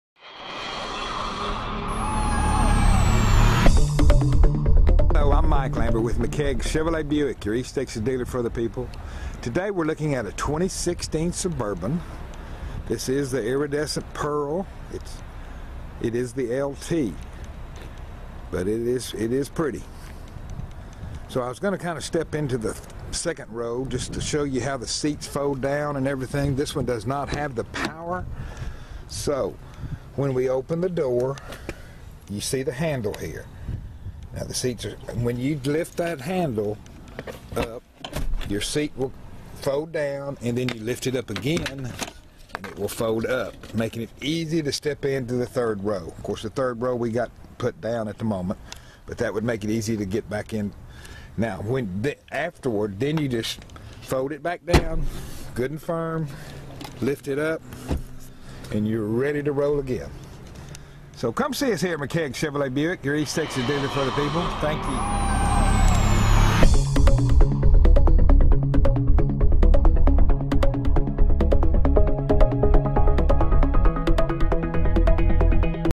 2016 Chevrolet Suburban Seat Function
Mike with Mckaig Chevrolet Buick in Gladewater, Texas gives us an overview on the 2cd row function in the 2016 Chevrolet Suburban.